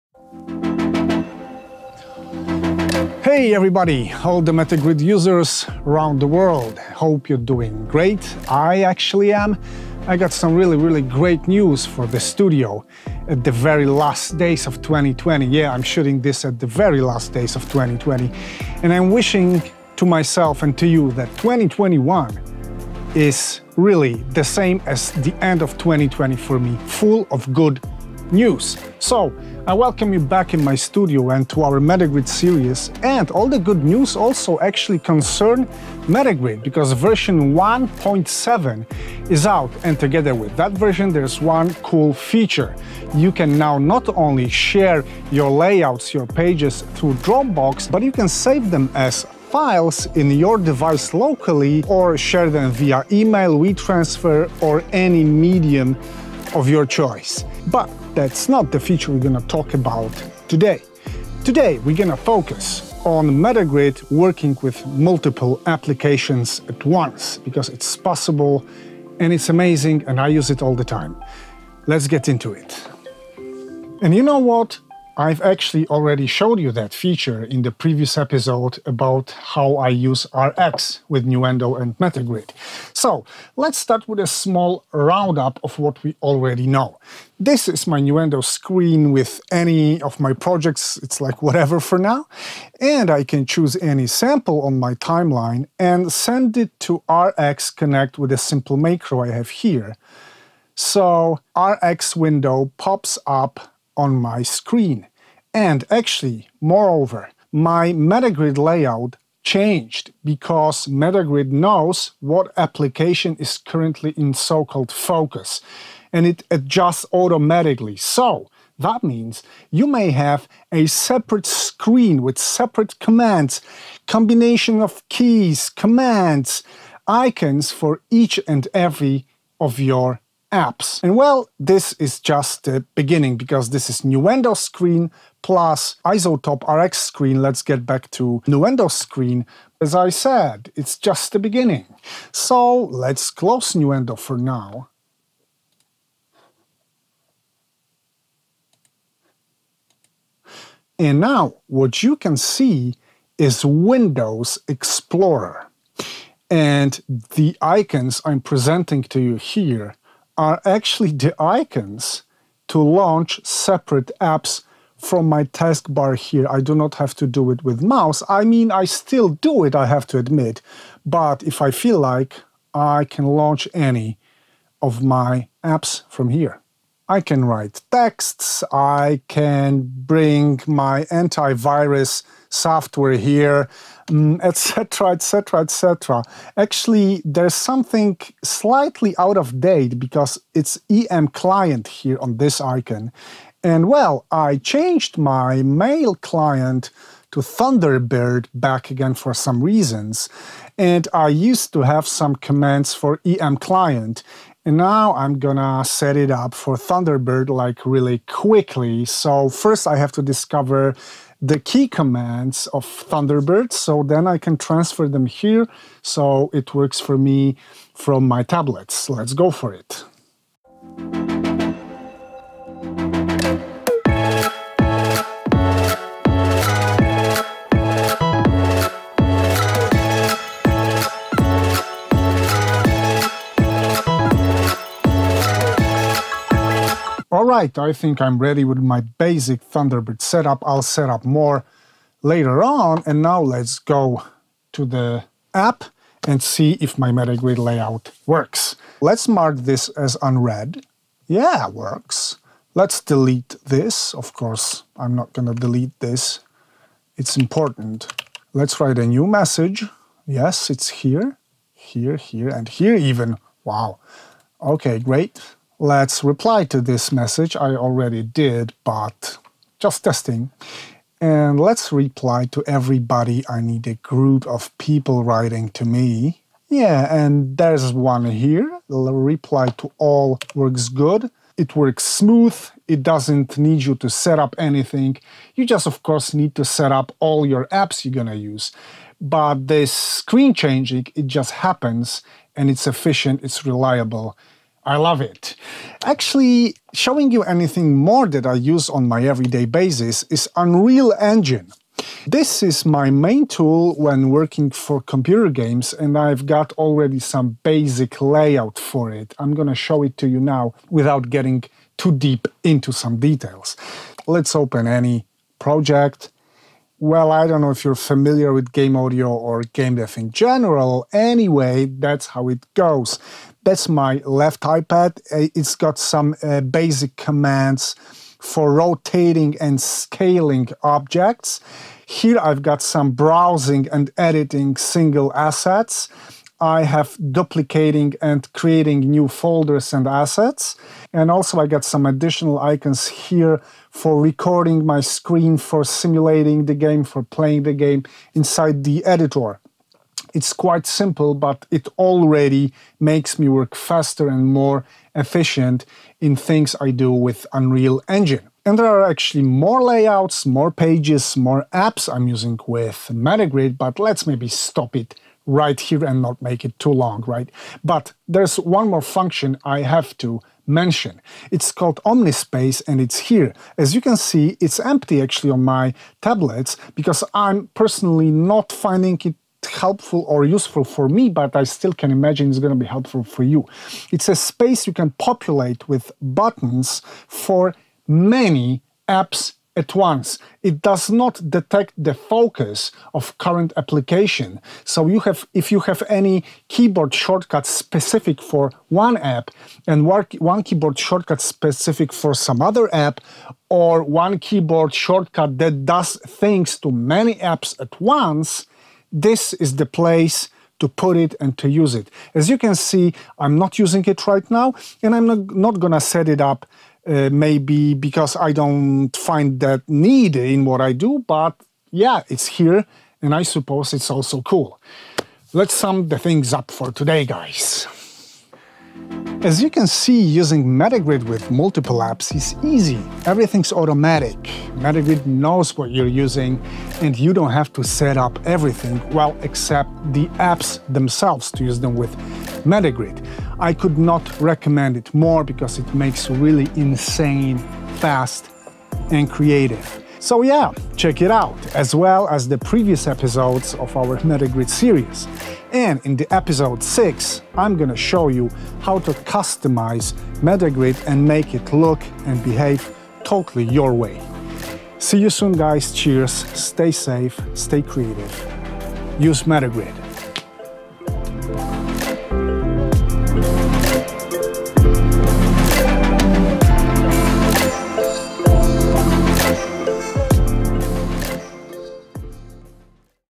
MetaGrid Classic with Paul #5: Controlling multiple applications
In this episode Paul shows how he uses Metagrid to control multiple applications to streamline his creative flow thanks to automatic application detection function in Metagrid.